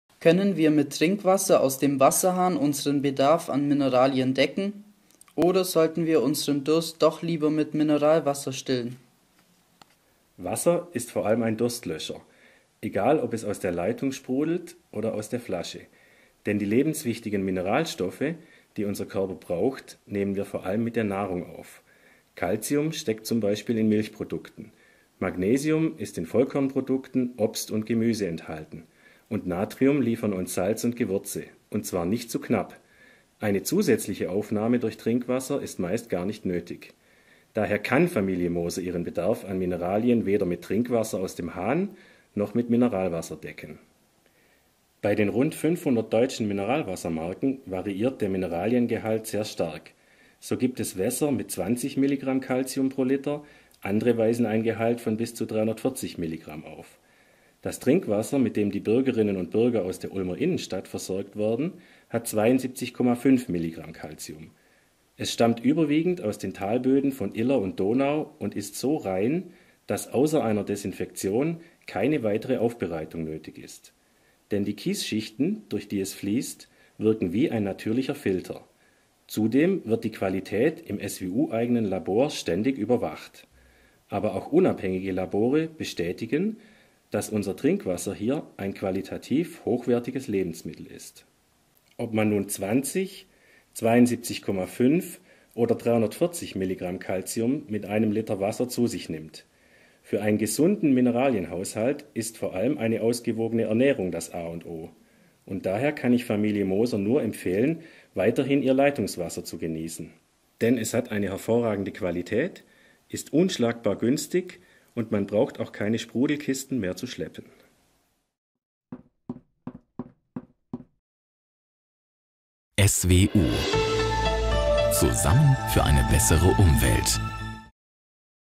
Gefragt. Gelöst. Podcast Folge 5
Familie Moser aus Ulm fragt sich: "Können wir mit Trinkwasser aus dem Wasserhahn unseren Bedarf an Mineralien decken? Oder sollten wir unseren Durst doch lieber mit Mineralwasser stillen?" Die Antworten auf diese und andere Fragen bei Gefragt. Gelöst. liefert ab sofort Markus Dilger.